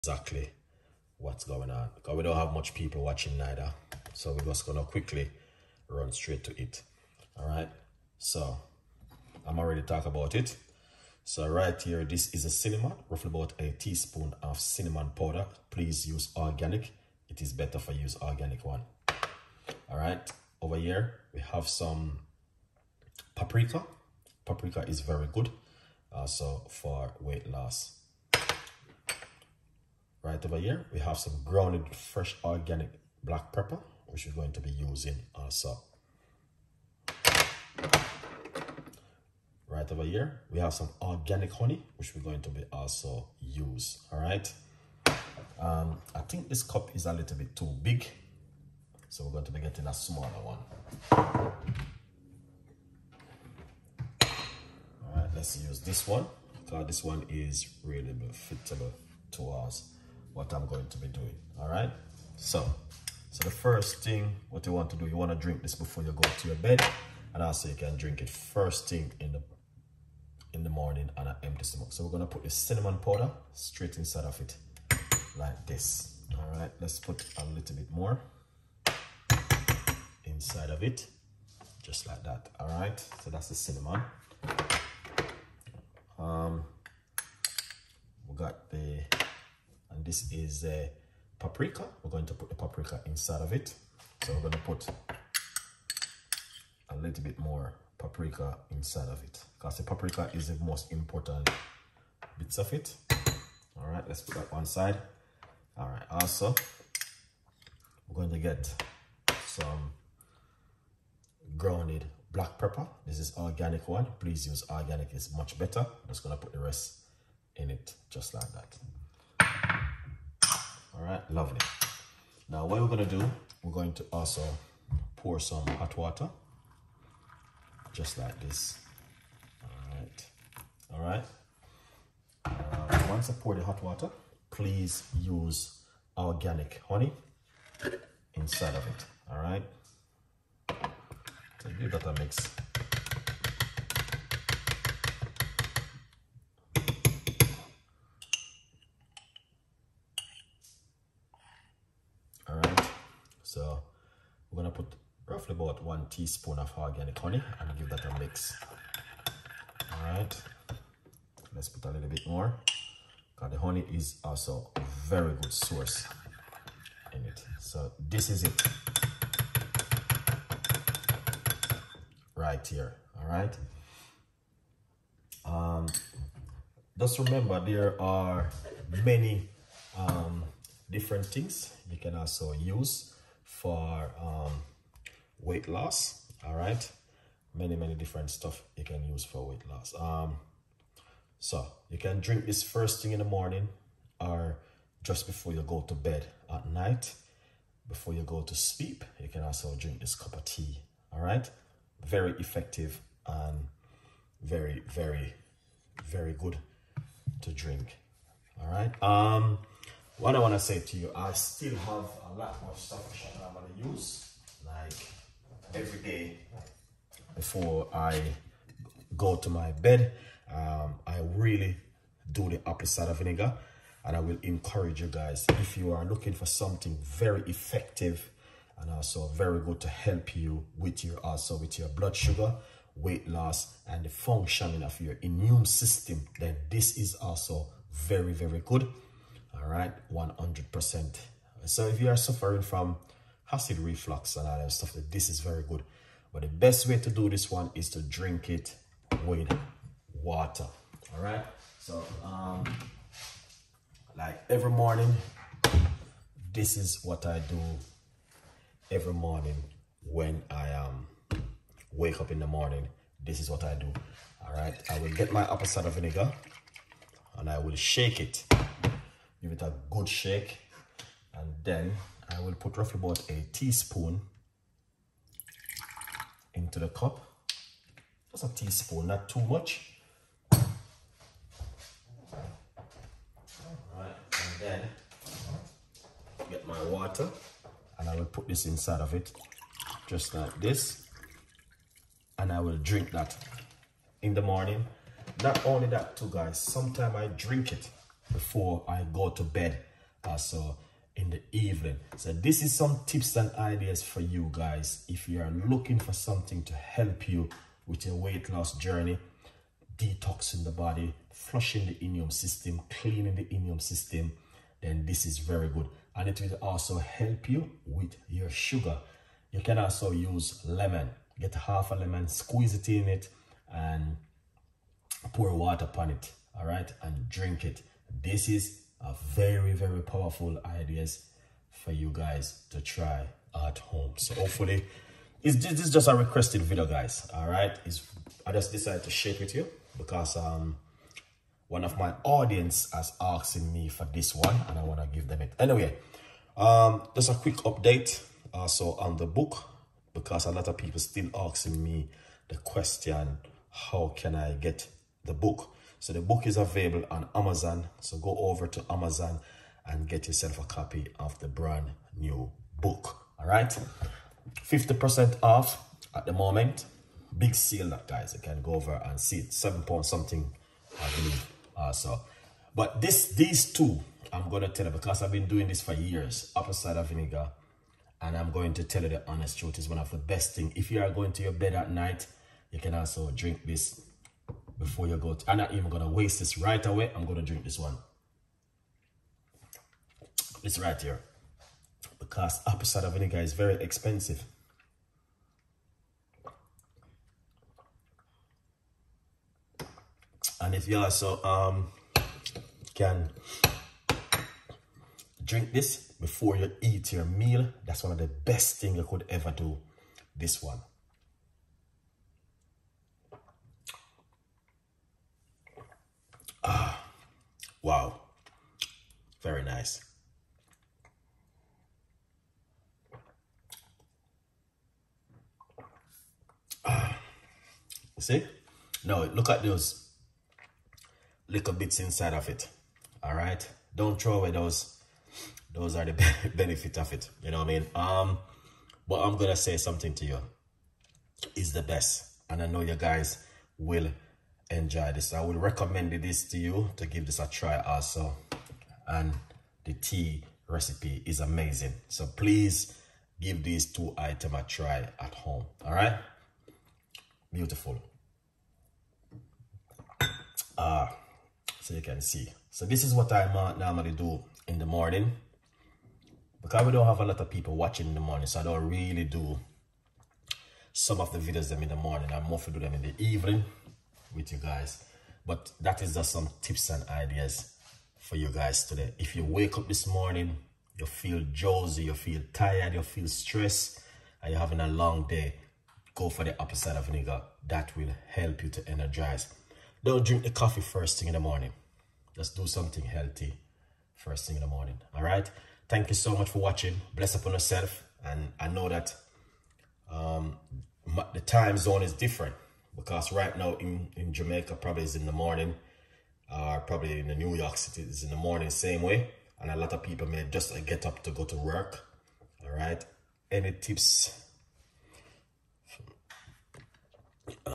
"1 Cup Before Bed: Sleep Better and Melt Belly Fat While You Dream!"
Beautiful cup of tea for weight loss and detoxing 1. Introduction: Start with a personal anecdote about a time when you struggled with sleep and weight management. Share how you felt tired, frustrated, and overwhelmed by the lack of results.

2. The Turning Point: Describe a moment of realization or a conversation with a friend or expert who introduced you to the idea of a special bedtime drink. You could mention how skeptical you were at first.
Ingredients
Cinnamon powder organic
Paprika powder, organic
Black pepper organic
Organic organic
Hot water